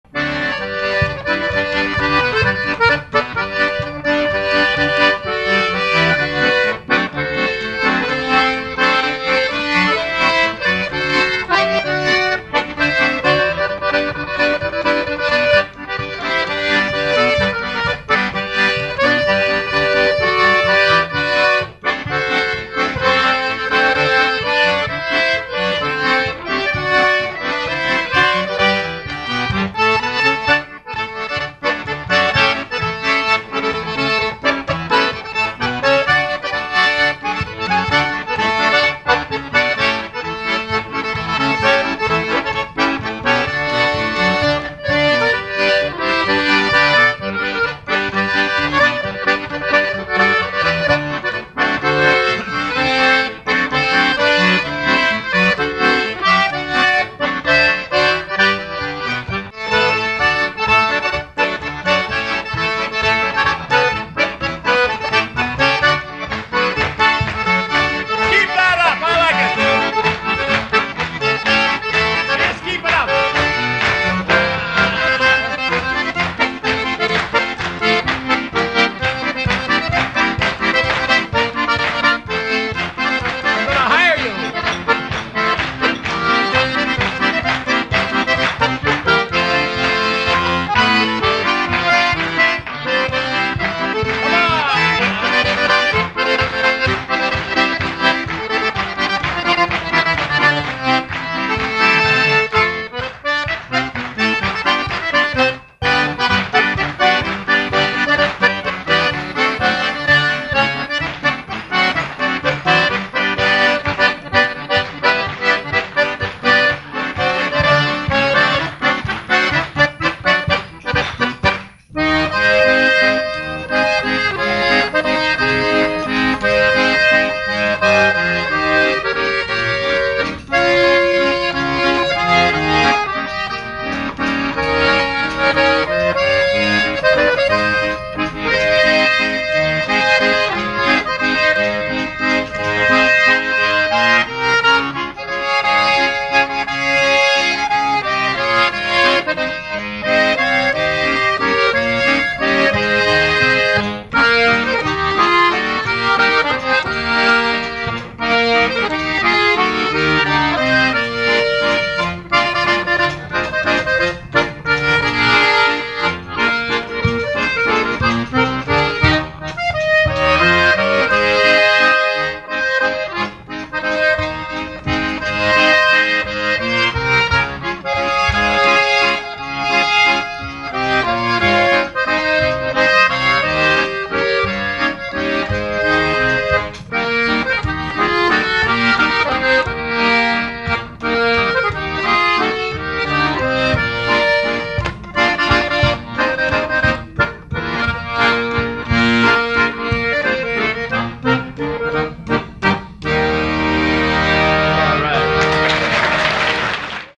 Walter Ostanek, 1992: Polka / Waltz Medley, Cleveland Style
Walter Ostanek, Canada's Polka King & multi-Grammy Award Winner, entertains the Toronto Accordion Club. Rarely does he perform solo without his band. It was fun to hear him unbanded (unplugged). One aspect of Cleveland style is playing the tune in octaves with the added 3rd or 6th. Currently, he is still very busy, following in the footsteps of his idol, the legendary Frankie Yankovic. Walter played these dances separately at full length; I edited them together to make it shorter for YouTube.